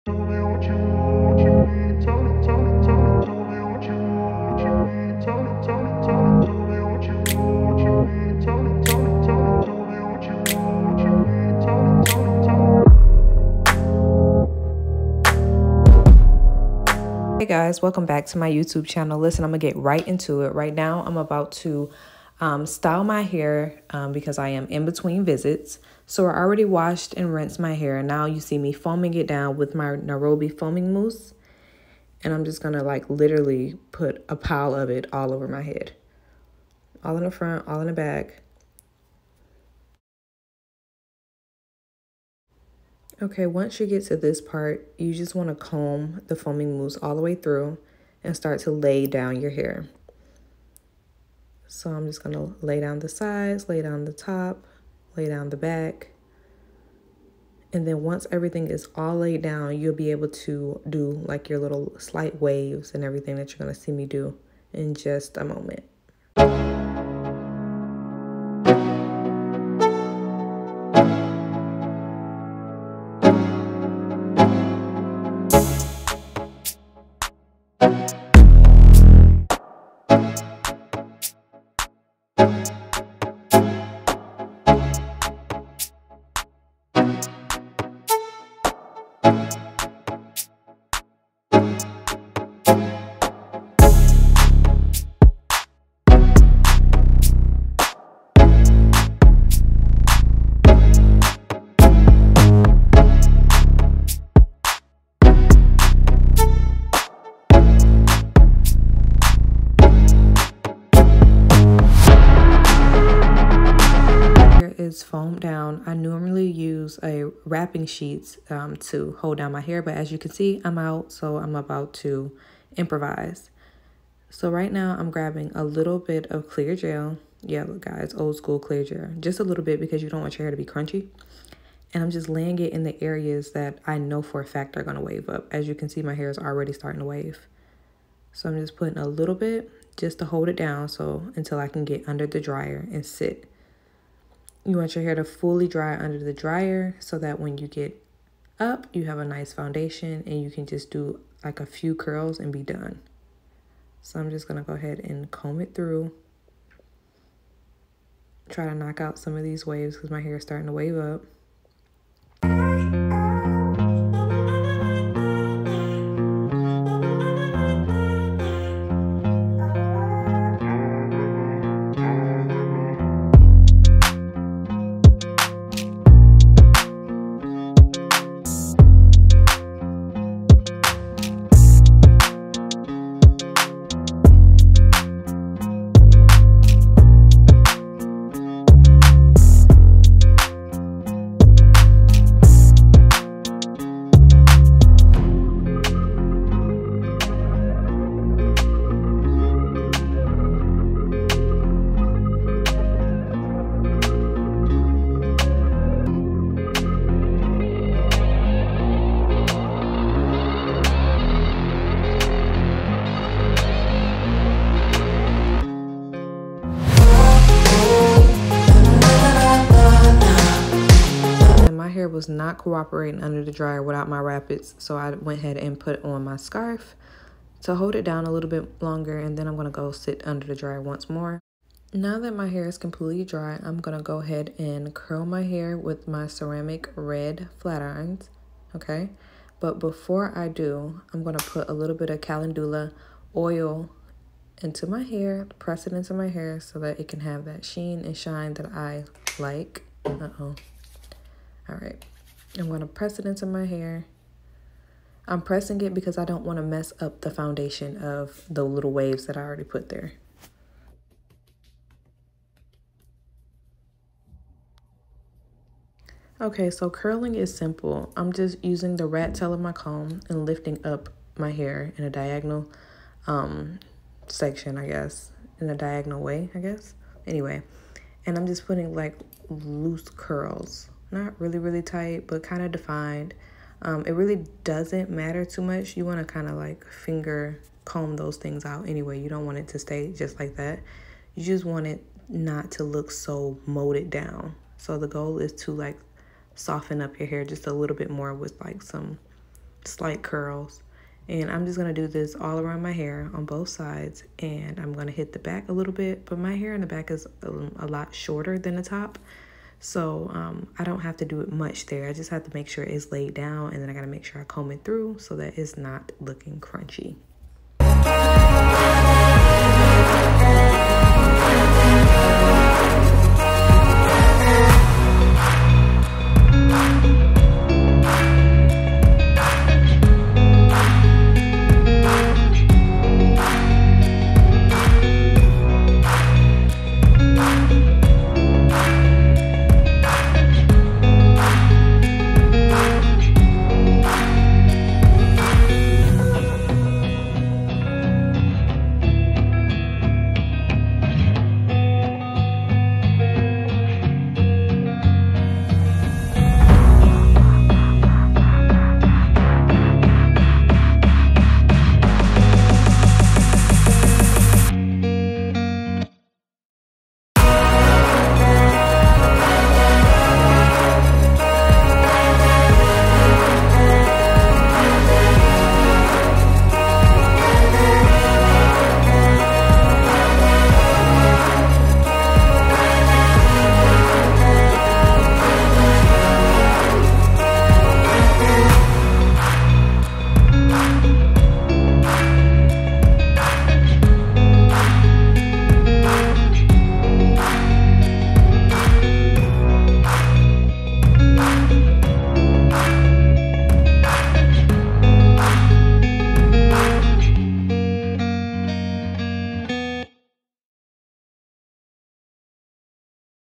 How to Style a Pixie Cut at Home| Beginner Friendly| Sky Atkins
Products used:
Nairobi Wrapping Foam
Clear Ice Gel
1" RED Flat Iron